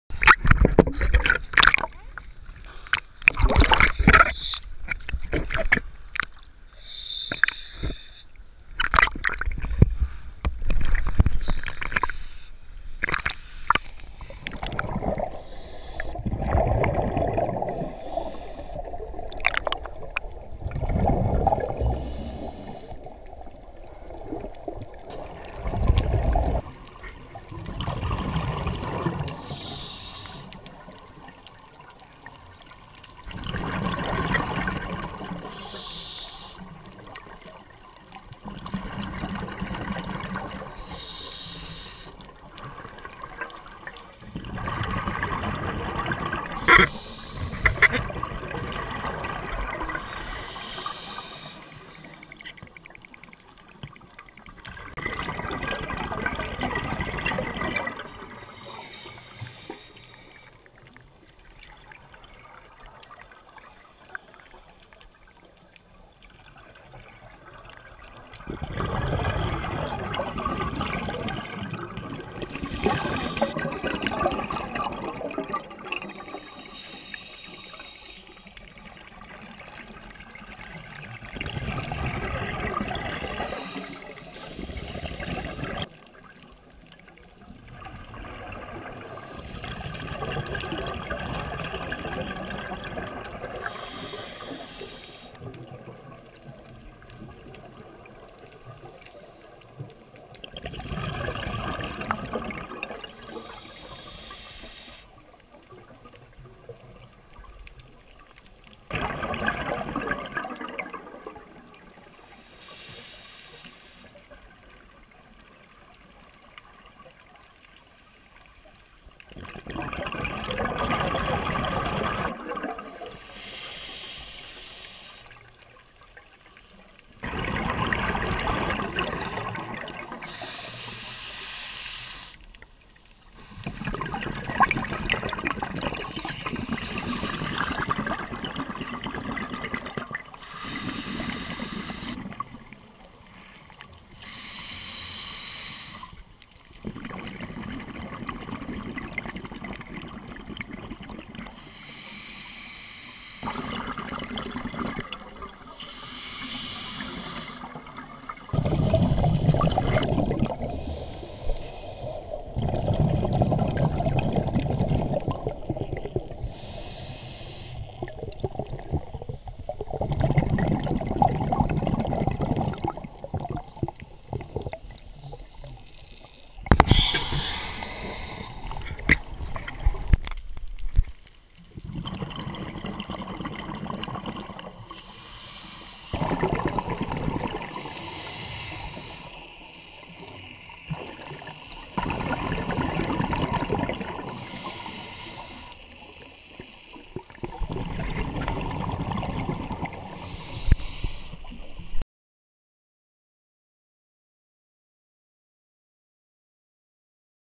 Final dive on the Open Water diver course, Blue Island Divers, Antiparos Greece
Blue Island Divers on Antiparos 2013. Hats off to the people at the dive centre. They rock!
BTW, this is the dive site called "Mick Jaggers lips".